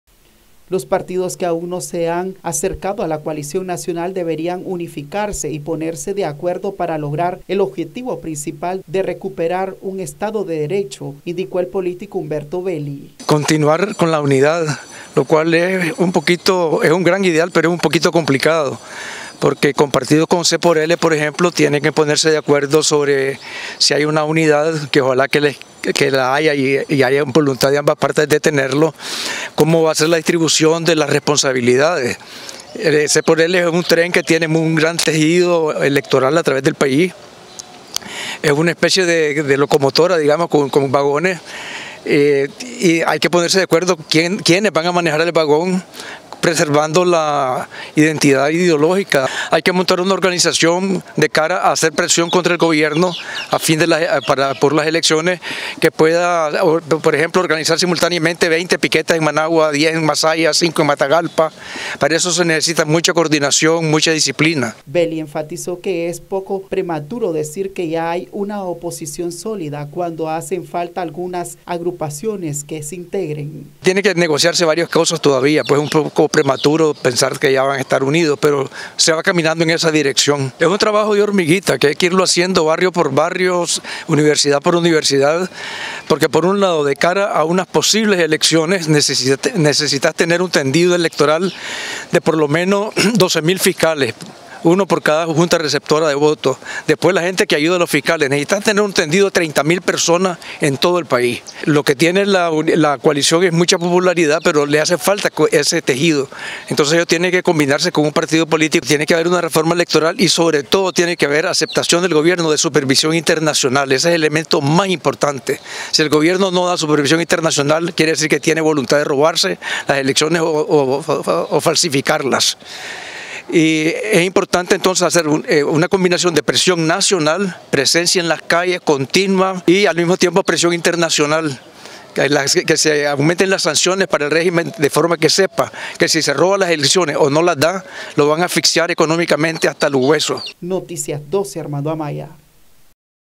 Belli: "Organizaciones políticas deben integrarse para formar una sola fuerza opositora"
El político Humberto Belli, dijo que las organizaciones políticas, deben integrarse para formar una sola fuerza opositora contra el actual gobierno.